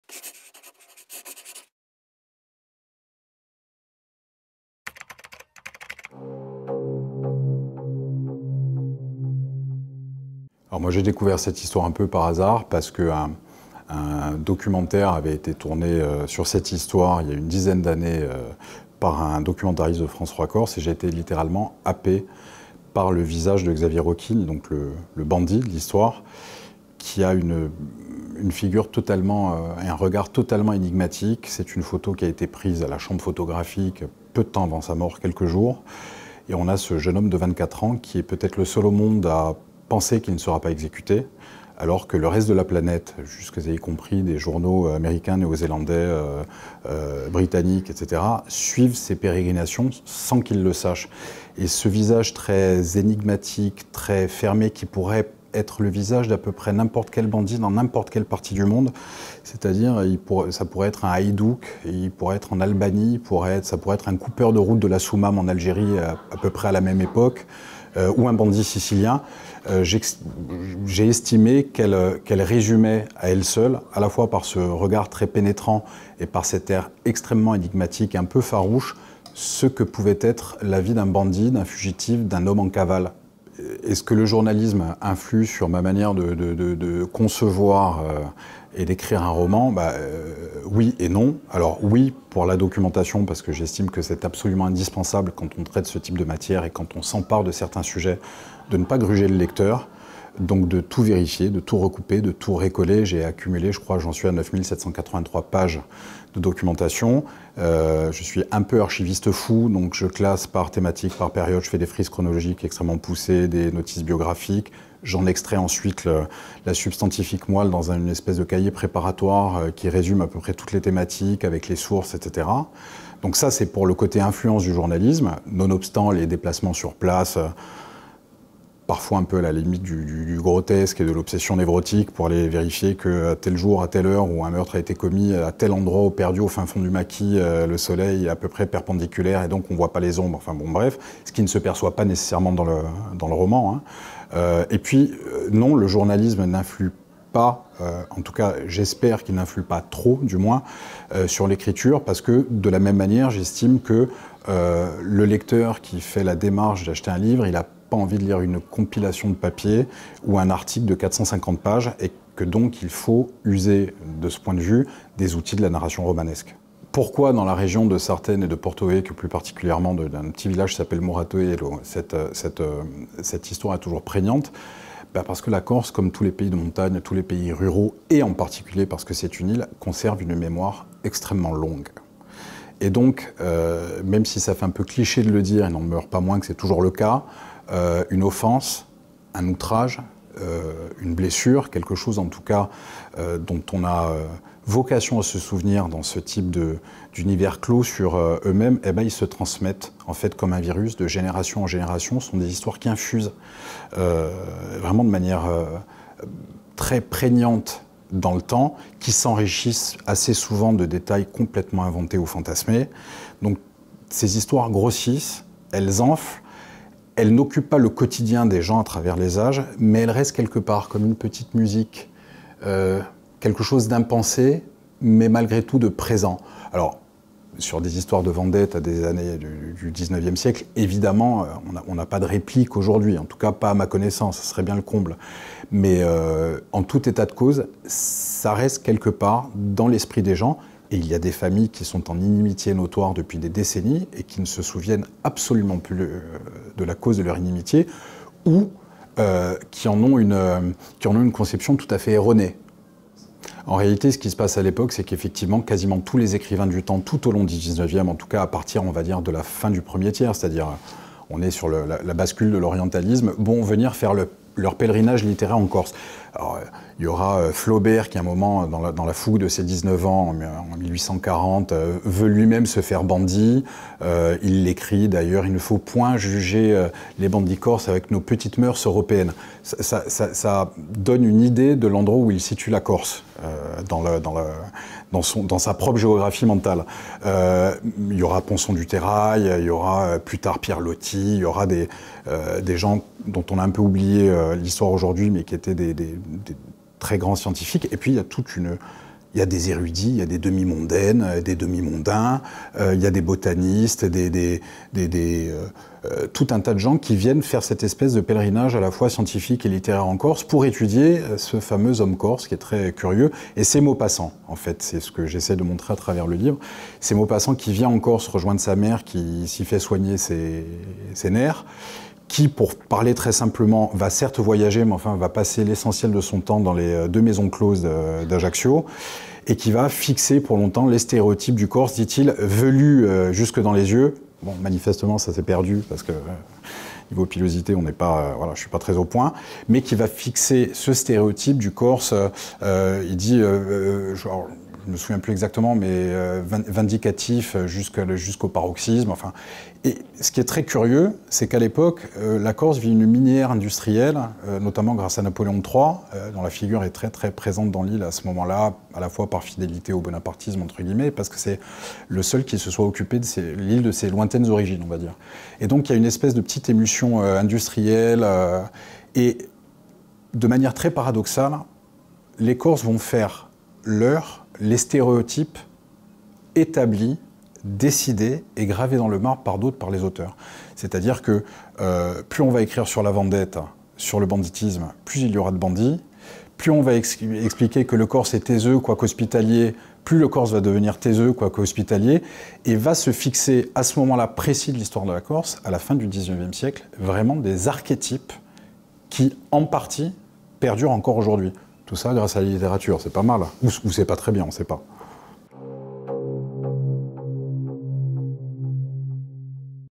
Antoine Albertini - Un très honnête bandit
Antoine Albertini vous présente son ouvrage "Un très honnête bandit" aux éditions JC Lattès. À paraître le 23 août 2023. Rentrée littéraire 2023.